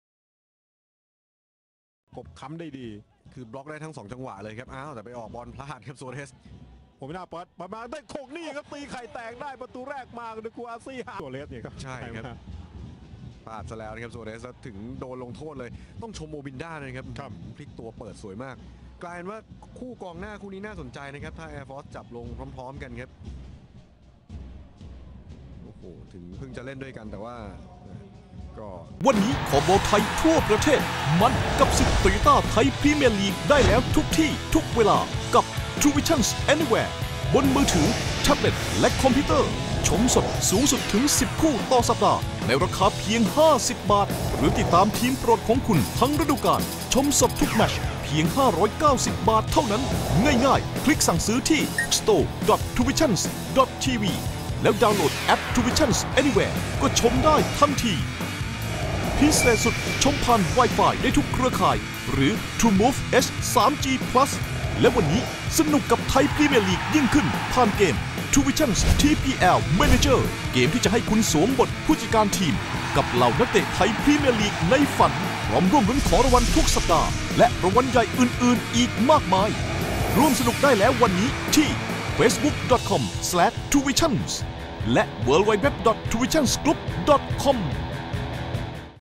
TPL 2014 - Highlight - PUFC 4-1 AFU (09-03-2014)
PUFC 1-0 AFU, PUFC 2-0 AFU, PUFC 3-0 AFU, PUFC 4-0 AFU, PUFC 4-1 AFU
นาทีที่ 83   โคอุสซี เฮร์มันน์    AFU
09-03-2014